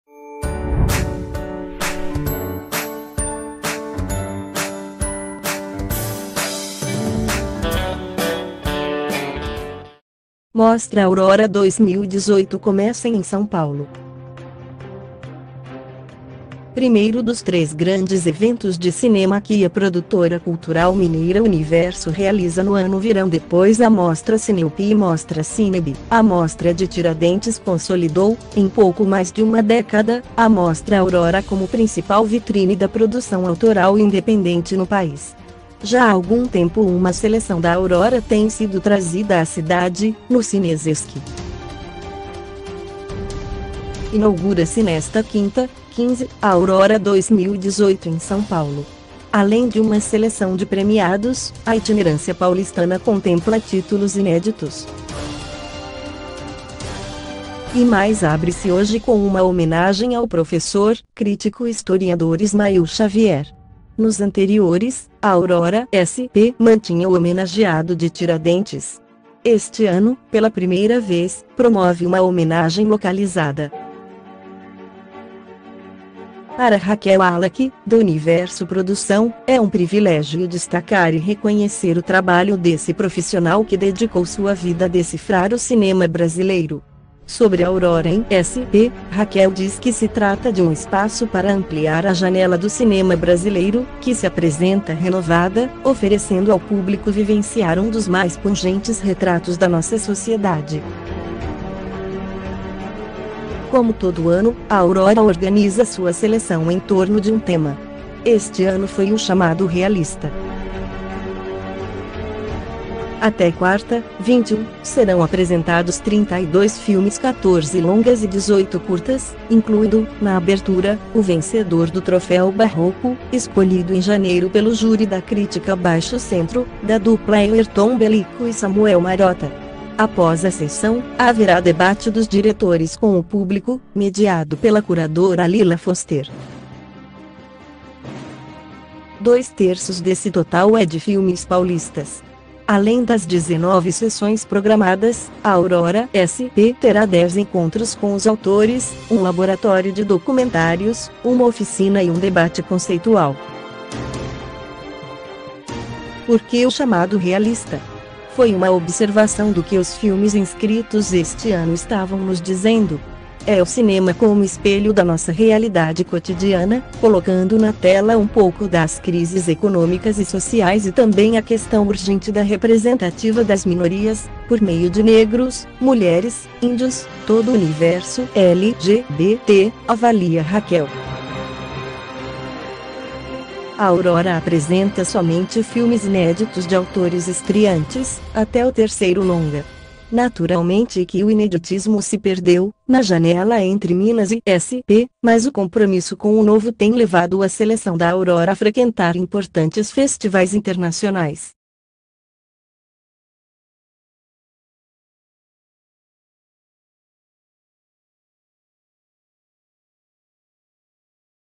Mostra Aurora 2018 começa em São Paulo
Primeiro dos três grandes eventos de cinema que a produtora cultural mineira Universo realiza no ano - virão depois a Mostra CineOP e Mostra CineBH -, a Mostra de Tiradentes consolidou, em pouco mais de uma década, a Mostra Aurora como principal vitrine da produção autoral e independente no País. Já há algum tempo uma seleção da Aurora tem sido trazida à cidade, no CineSesc.